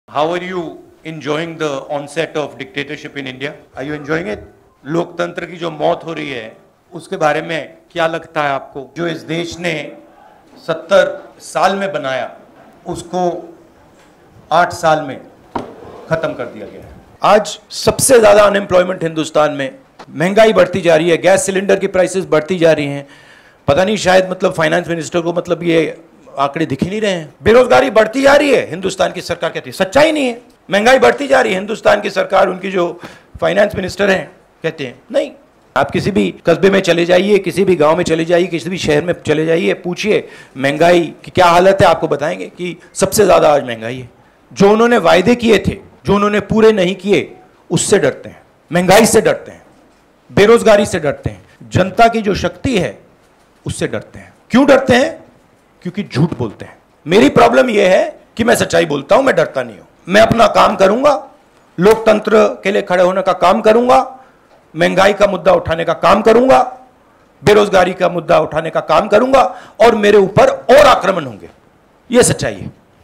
This is BJP’s ‘Amrit Kaal’! | AICC HQ | New Delhi
Not a single Indian has been spared by the 'double-engine' of backbreaking inflation and historic unemployment. This is BJP’s ‘Amrit Kaal’!
What India built brick by brick, for nearly 70 years, has been destroyed by their arrogance in just 8 years.